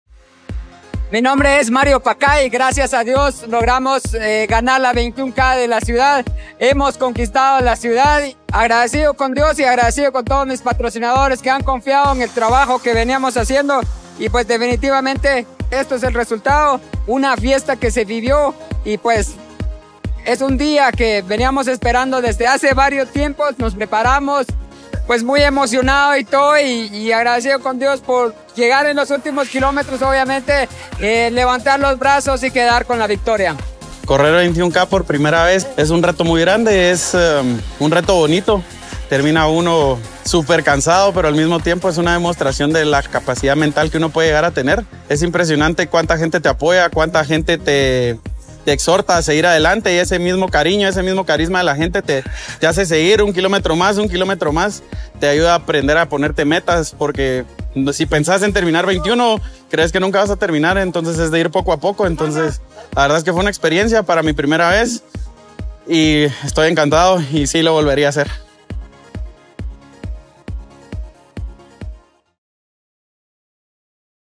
Se realiza la décimo cuarta edición de la 21K Ciudad de Guatemala
(Producción y edición de video: Soy502)